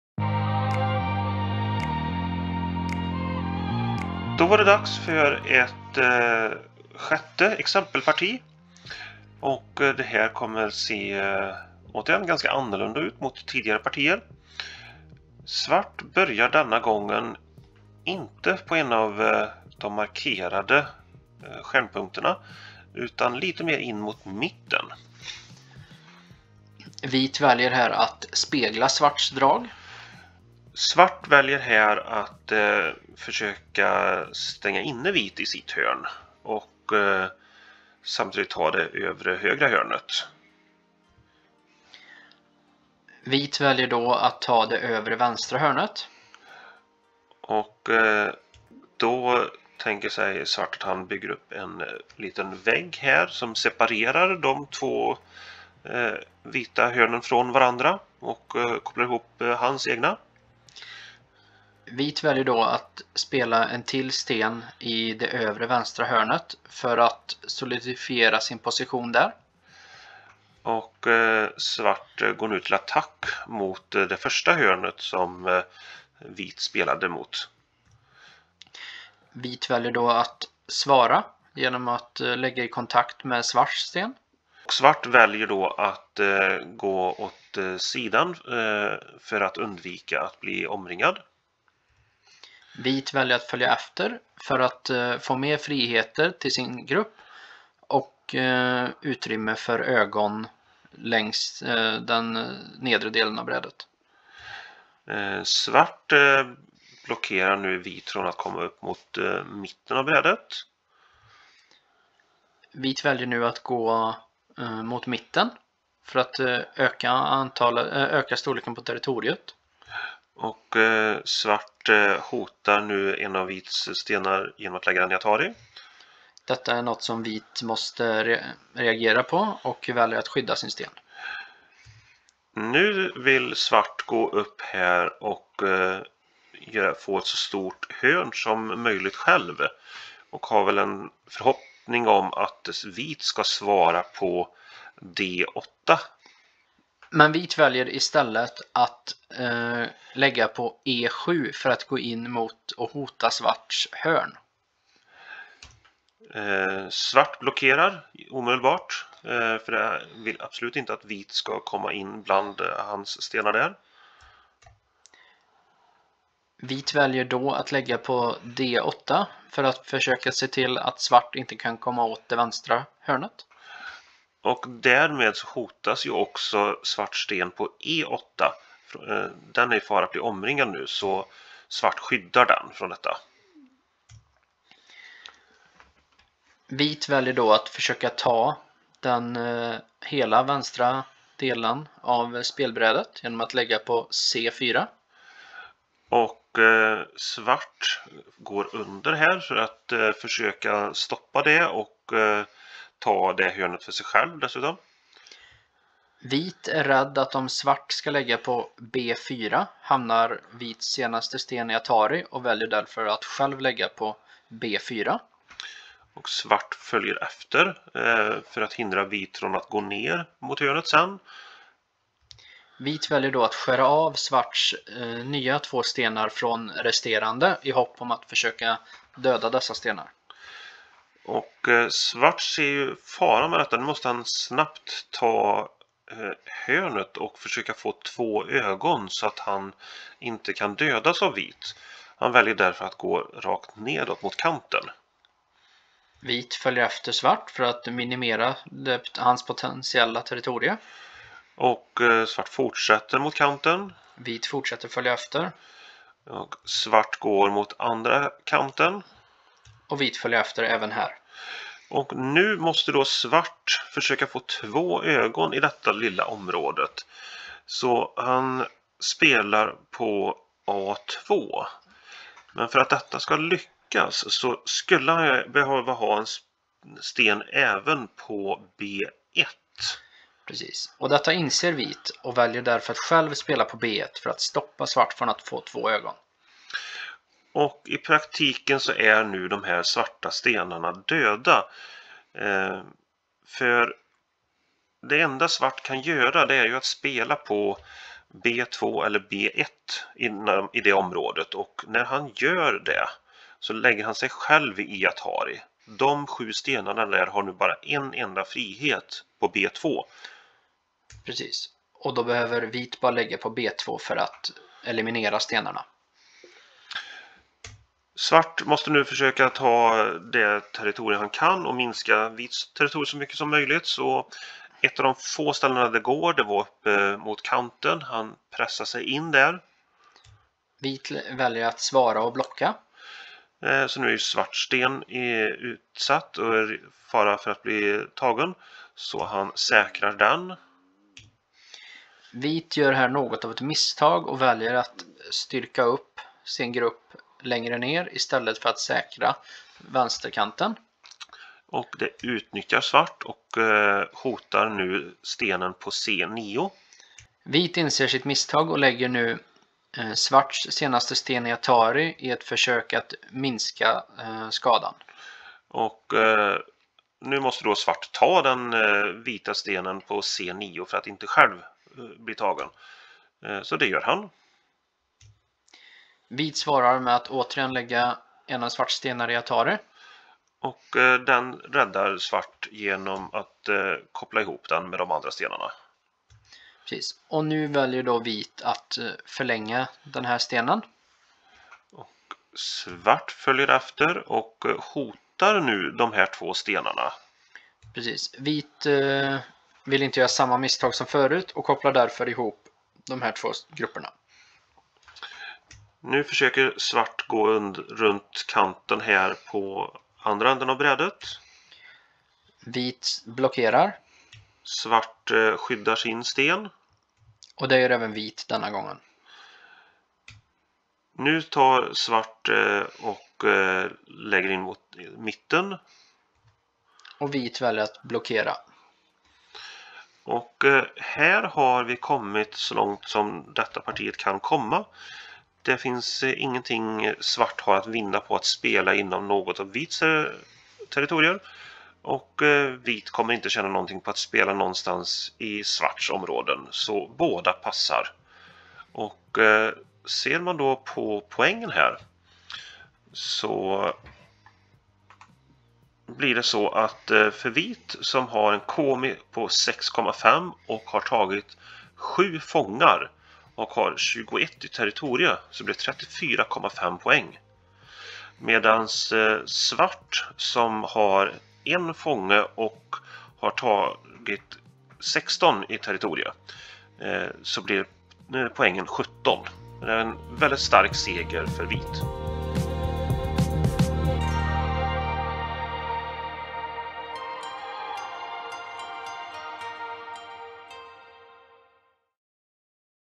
Del 6 Exempelparti Go litet bräde 9x9
Petter och Ola går igenom en serie partier som är typiska för hur nybörjare ofta spelar, del 6.

Många nybörjare upplever det som svårt att lära sig av att se på exempelpartier av mer erfarna spelare för det försiggår så mycket som de inte har bemästrat än. De här partierna hålls avsiktligt väldigt enkla för att du ska få chansen att ta till dig saker i ett lugnare tempo.

Besök oss på gobutiken.se